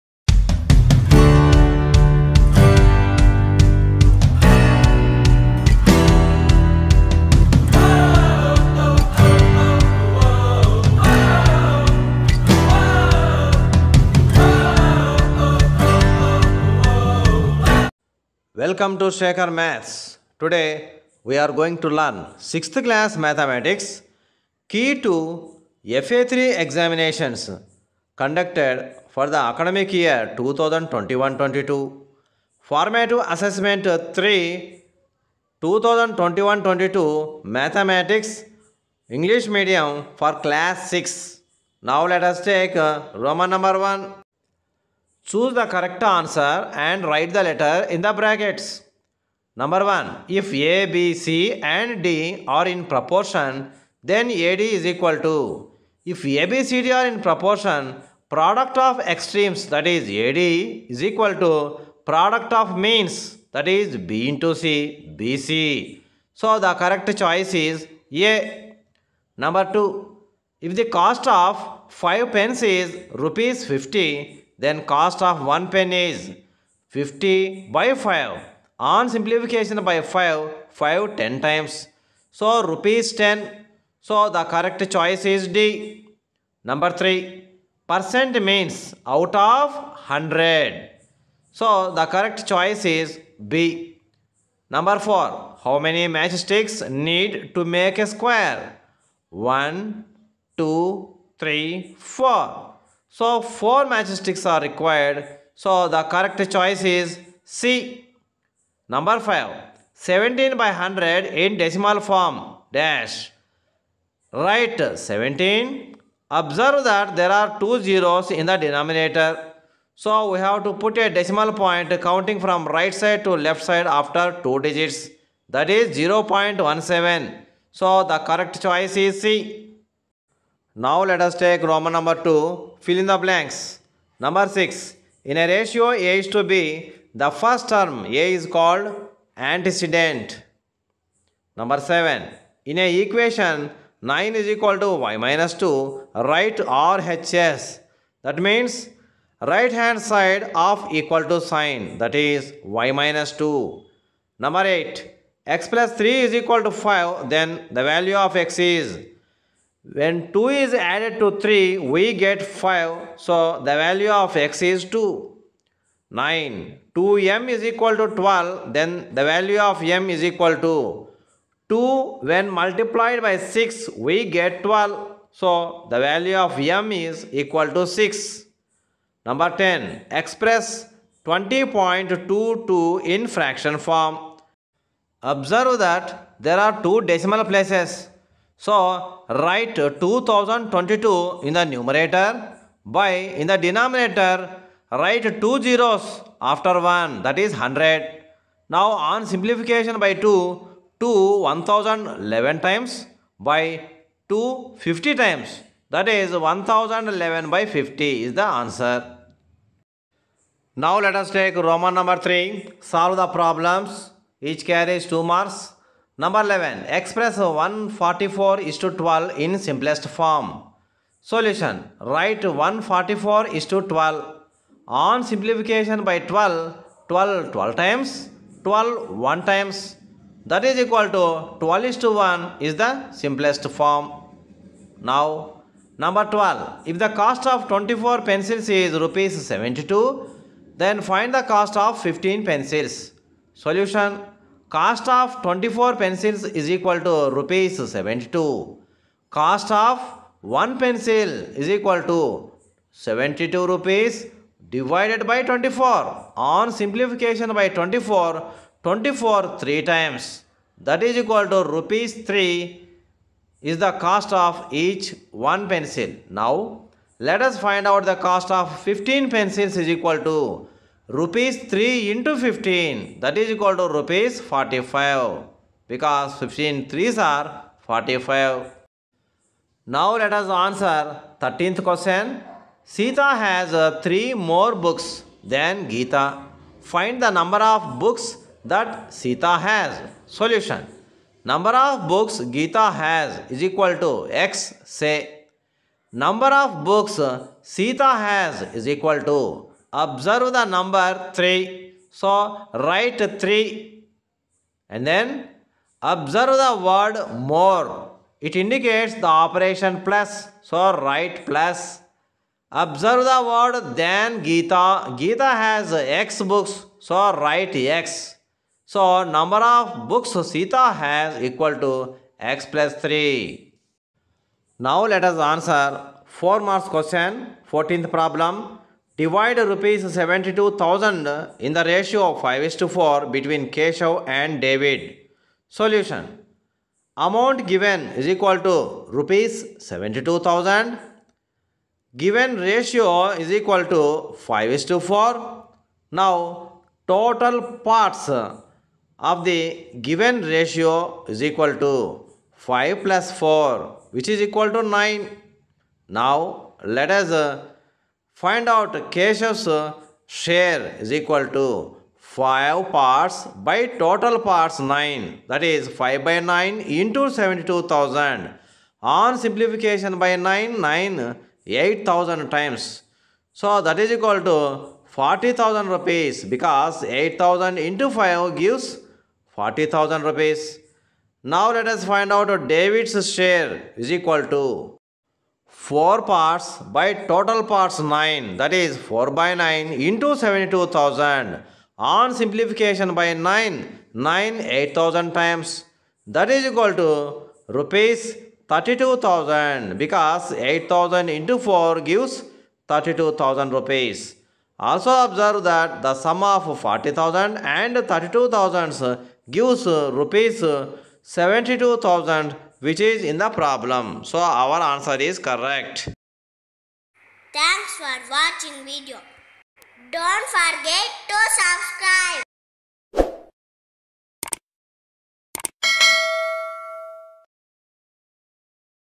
6th || Key To FA3 (2021-22)
Solutions to the problems of FA3 Exams Of AP 6th class Mathematics held for the academic year 2021-22.